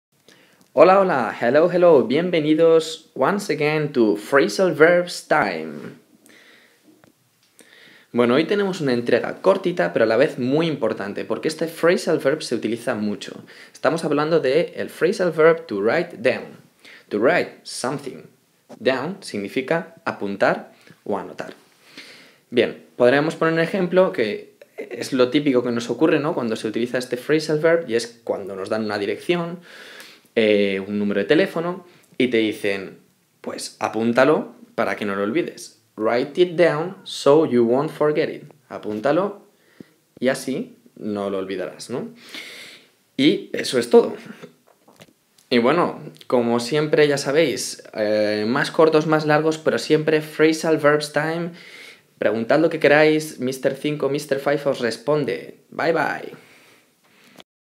PHRASAL VERBS #13 : WRITE DOWN. APRENDE, PRACTICA Y MEJORA TU INGLÉS CON MISTERCINCO
Video 13 de la serie de Mistercinco para aprender y practicar con los Phrasal Verbs. En esta ocasión: TO WRITE DOWN

Recuerda también que puedes enviarnos todas tus dudas o problemas que necesites resolver a nuestra página web

MisterCinco, el rey del aprobado.